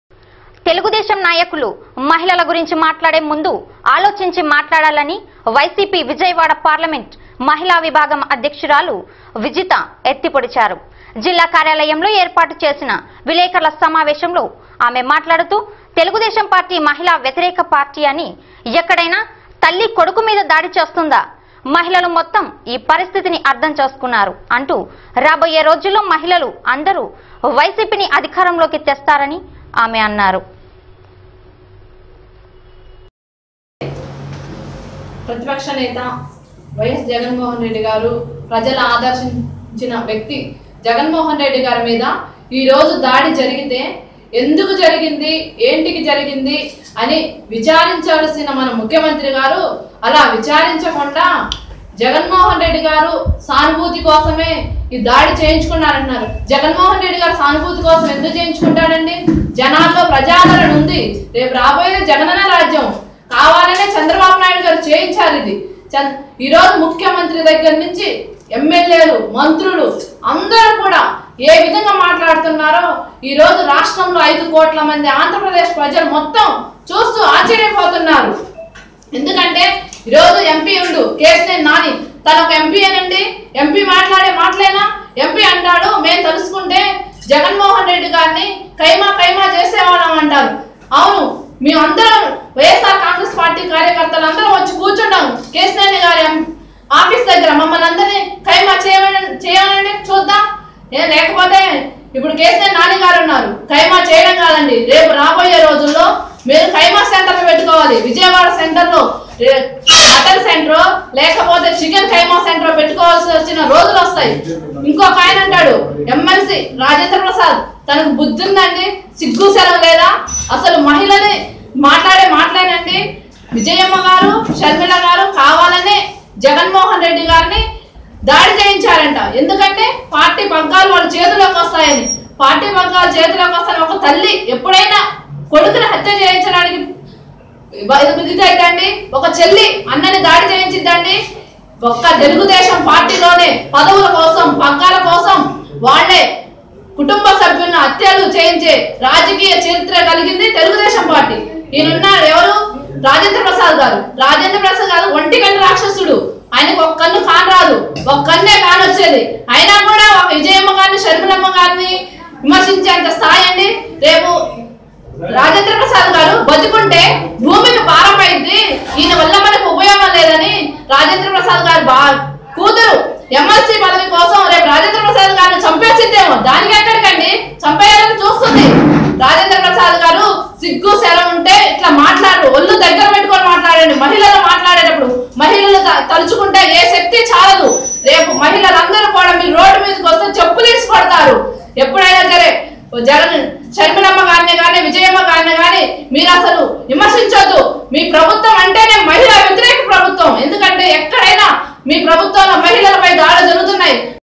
తెలుగుదేశం పార్టీ మహిళా వ్యతిరేక పార్టీ .. పార్లమెంట్ మహిళా విభాగం అధ్యక్షురాలు విజిత // NSR NEWS
తెలుగుదేశం నాయకులు మహిళల గురించి మాట్లాడేముందు ఆలోచించి మాట్లాడాలి అని వైసీపీ విజయవాడ పార్లమెంట్ మహిళా విభాగం అధ్యక్షురాలు విజిత ఎత్తి పొడిచారు. జిల్లా కార్యాలయంలో ఏర్పాటు చేసిన విలేకరుల సమావేశంలో ఆమె మాట్లాడుతూ "తెలుగుదేశం పార్టీ మహిళా వ్యతిరేక పార్టీ అని ఎక్కడైనా తల్లి కొడుకు మీద దాడి చేయిస్తుందా? మహిళలు మొత్తం ఈ పరిస్థితిని అర్ధం చేసుకున్నారు. అంటూ  రాబోయే రోజుల్లో మహిళలు అందరూ వైసీపీ ని అధికారంలోకి తెస్తారని ఆమె అన్నారు.